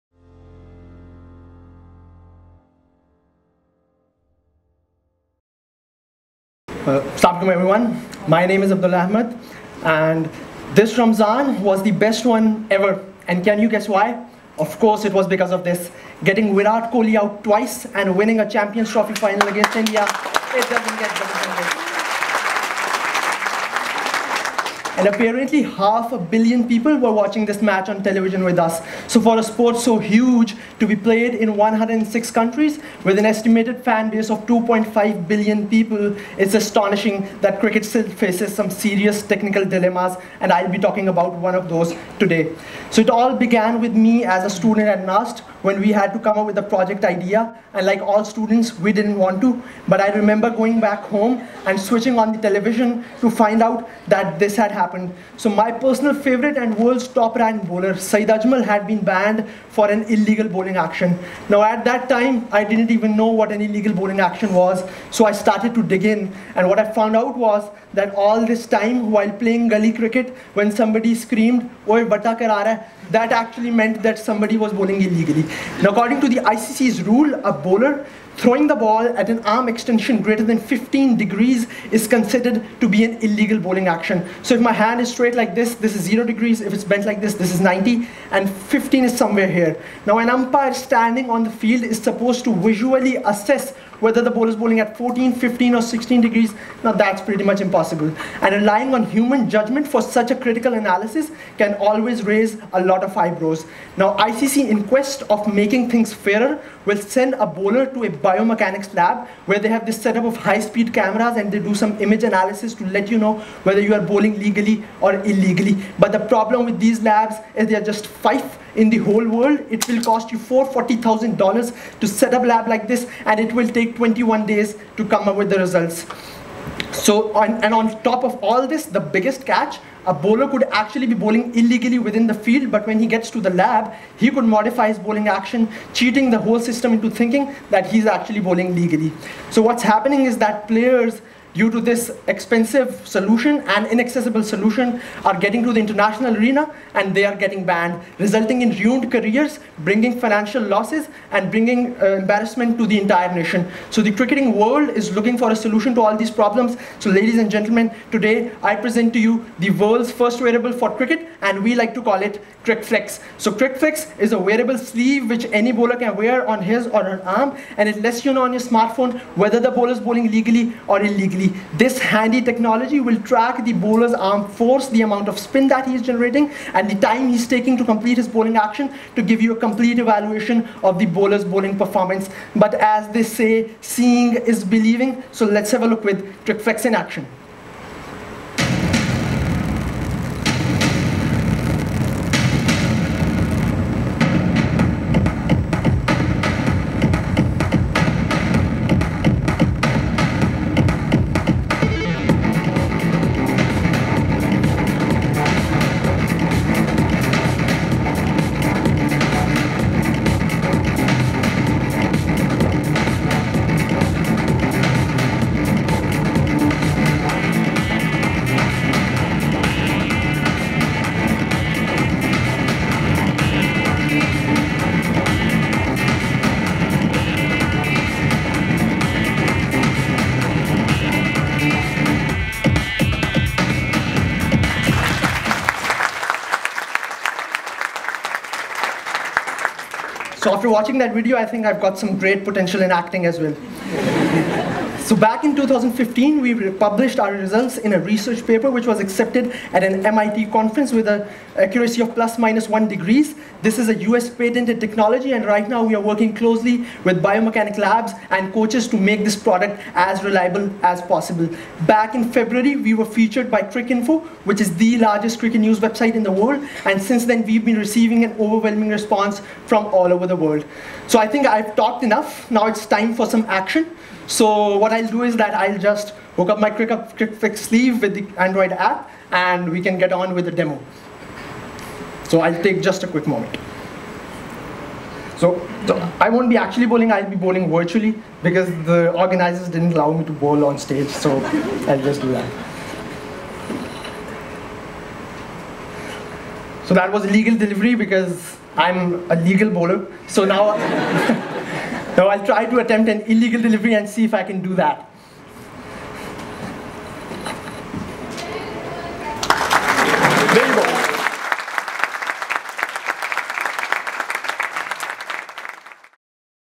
From “Wataa” Bowler to “What a” Bowler: Wearable Tech in Cricket | Abdullah Ahmed | TEDxITU
As a young engineering student, Abdullah was inspired to create CricFlex - a wearable sleeve that helps bowlers maintain a legal bowling action – after years of frustration over controversies surrounding illegal bowling actions. His talk focuses on the importance and need for the functional wearable sleeve and its impact on international cricket.  Founder and CEO of CricFlex, Abdullah Ahmed is a Pakistani innovator, researcher and entrepreneur. Abdullah has three research publications in peer-reviewed IEEE conferences with his latest research presented at MIT, USA. For his achievements, Abdullah has been awarded Prime Minister's Bronze Medal, Rector's Gold Medal in academia and Pakistan's Innovator of the Year Award, 2015. He is also a Rajeev Circle Fellow. This talk was given at a TEDx event using the TED conference format but independently organized by a local community.